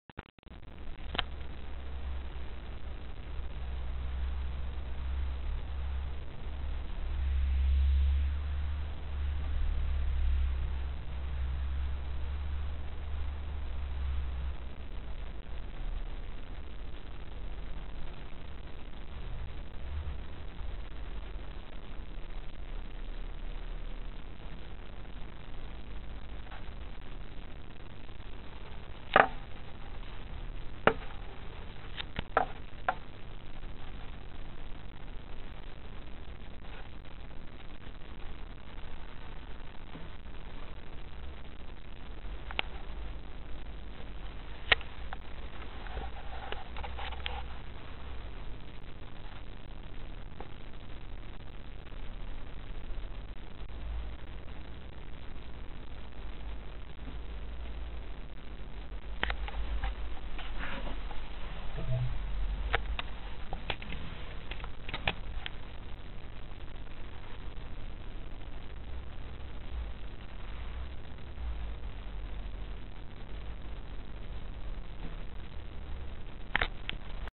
DSCI1037
Wellwater - der Hahn läuft aus dem Inneren heraus aus.